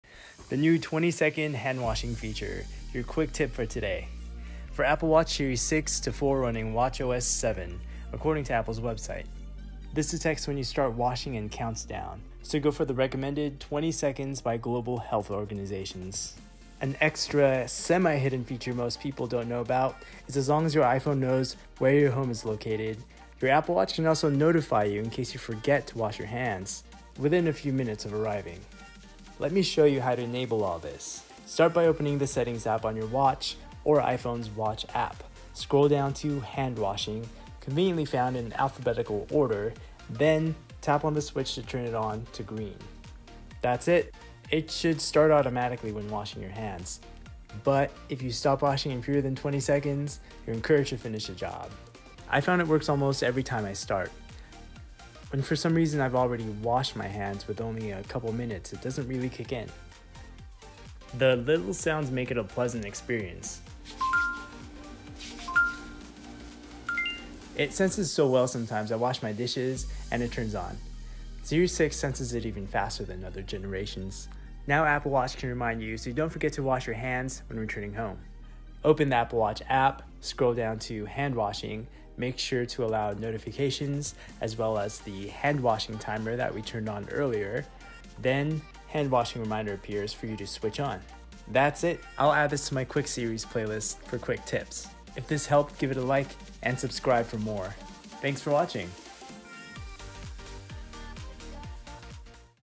How to ENABLE 20 second HANDWASHING & BE REMINDED on APPLE WATCH Series 6, 5, 4
How to Turn on Handwashing and be reminded with Notification after minutes of arriving home

This detects when you start washing and counts down so you go for the recommended
 20 seconds by global health organizations.

Find out how to enable both features in this quick, minute and a half video